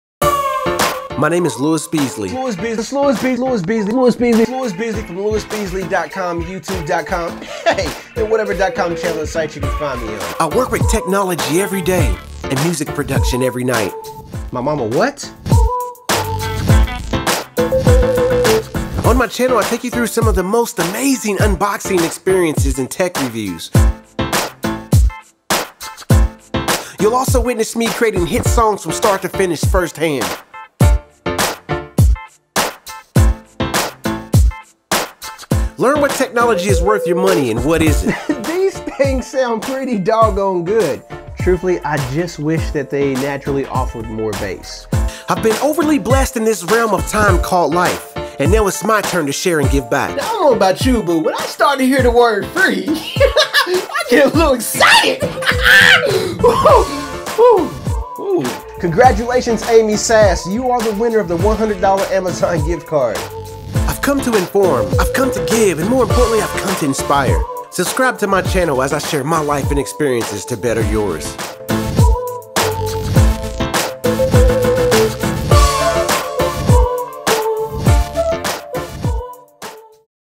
Lewis Beasley's Channel Trailer
Hello! This is a the trailer for my Youtube channel. On my channel you'll find quality and informative tech reviews,  unboxings, and music. For more random things, follow me on the below social networks.